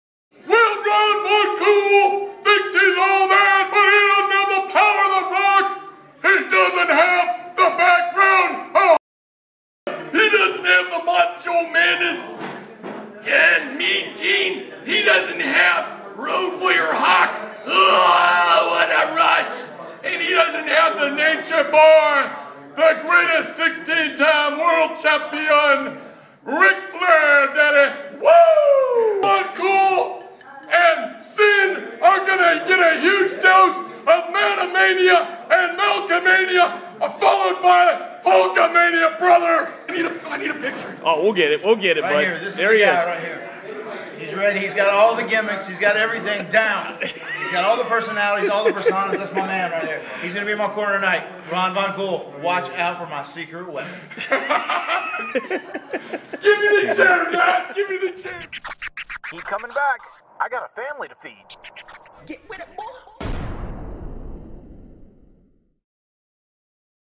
Matt Meets Malcolmania!
While in Newark, CA, I met a pro wrestling superfan named Malcolm-and Malcamania ran wild! Check out my man Malcolm as he stood tall in my corner and had my back that night at BTW.